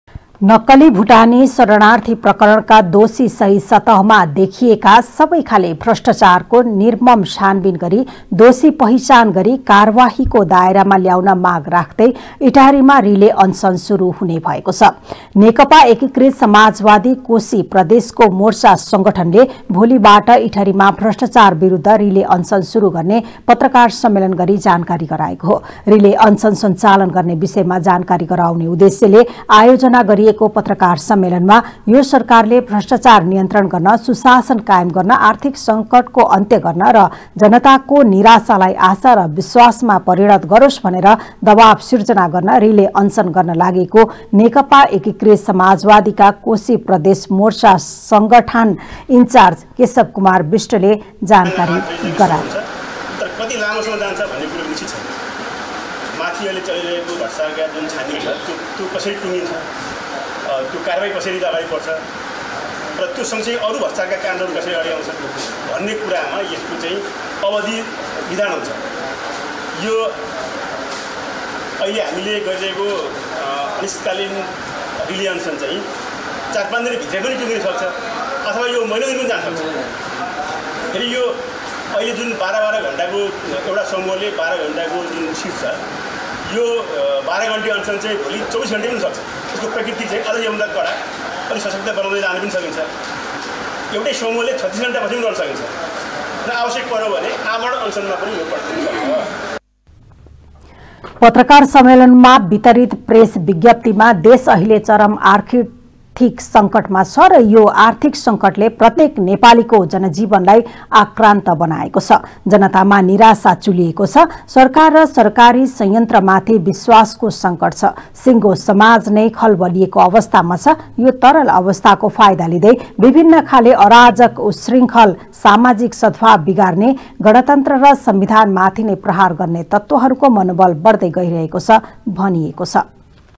भ्रष्टचारको निर्मम छानबिन गरी दोषी पहिचान गरी कारवाहीको दायरामा ल्याउन माग राख्दै इटहरीमा रिले अनसन
नक्कली भुटानी शरणार्थी प्रकरणका दोषी सहित  सतहमा देखिएका  सबै खाले भ्रष्टचारको निर्मम छानबिन गरी दोषी पहिचान गरी कारवाहीको दायरामा ल्याउन माग राख्दै इटहरीमा रिले अनसन सुरु हुने भएको छ ।